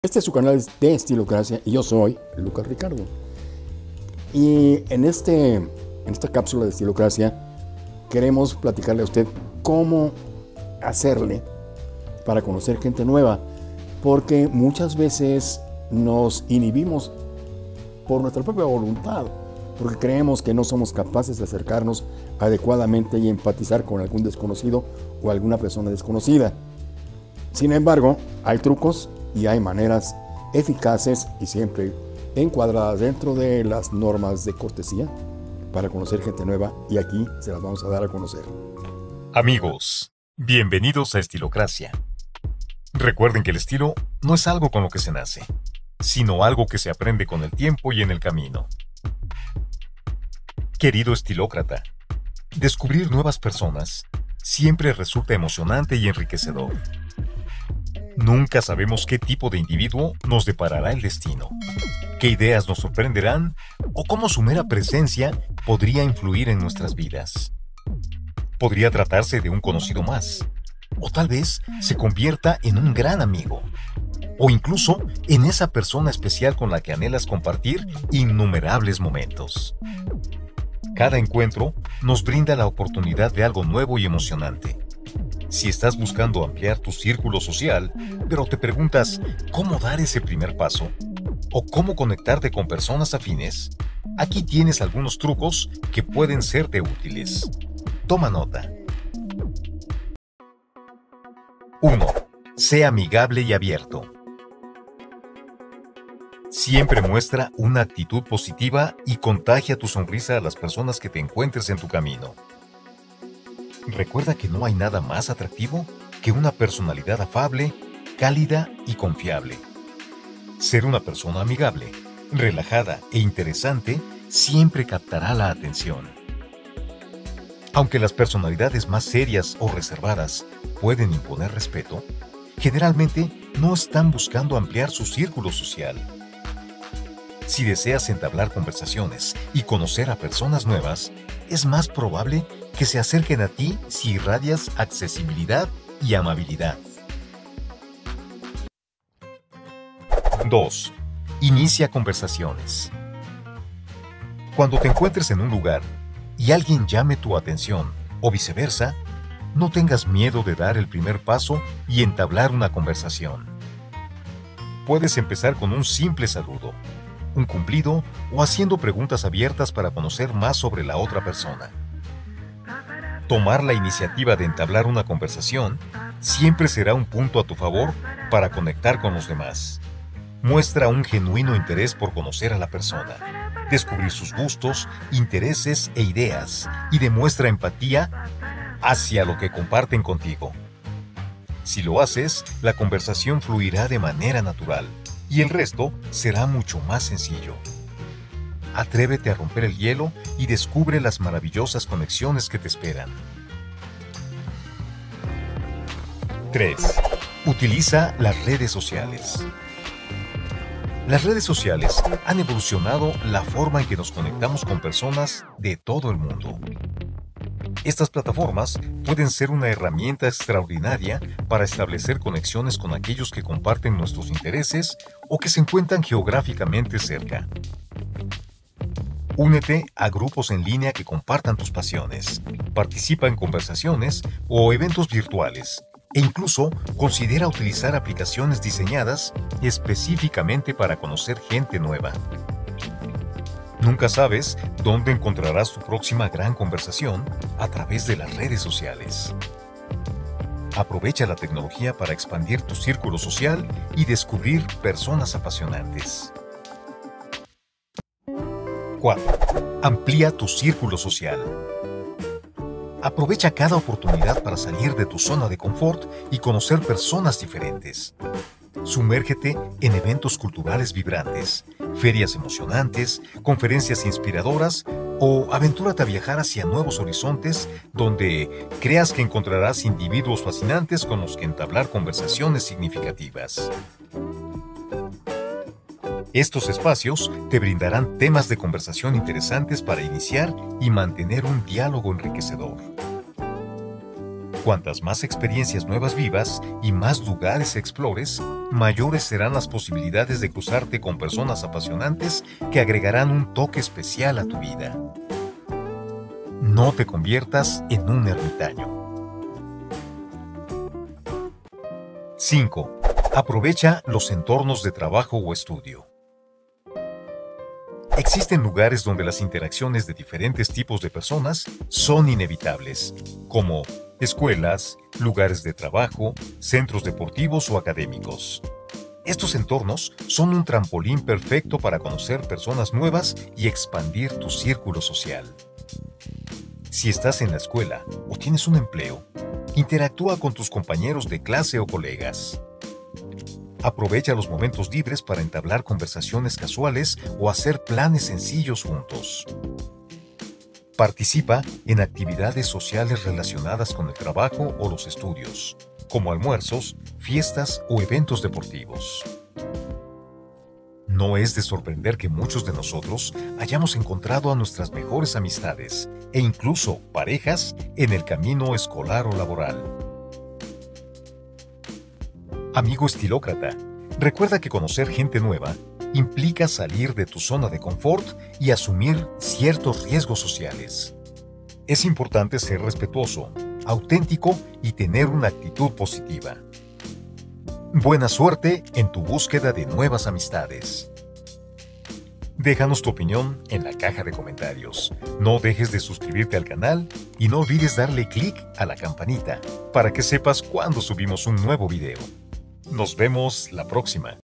5 trucos PARA CONOCER gente NUEVA 👫 !!!
🤔¿QUIERES conocer GENTE nueva pero NO sabes CÓMO? PON atención a los SIGUIENTES trucos 😉!!!

00:00 Introducción
01:40 Sé amigable y abierto
02:29 Inicia conversaciones
03:26 Utiliza las redes sociales
04:19 Amplia tu circulo social
05:12 Aprovecha tus entornos

Visita nuestro sitio asociado de Amazon para productos de tu interés: